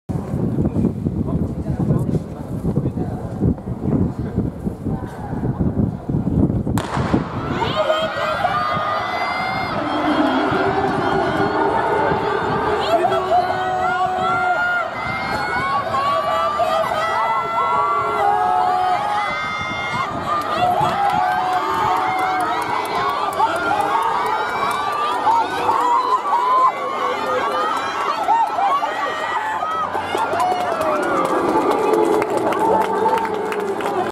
女子200m 決勝
-0.2
青山 23"81
中村 24"00
江口 24"06
小山 24"17
北澤 24"21
山本 24"46
土井 24"59
木村 24"89